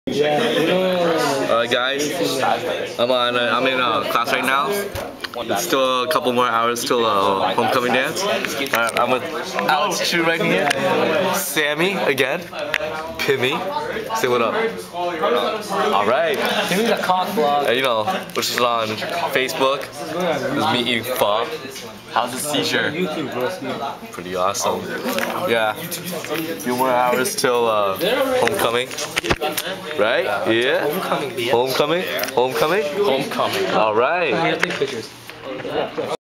Vlog: Homecoming dance part 2
Just my high school day up until the homecoming dance was over. Get a little bit of knowledge of how my life at school is.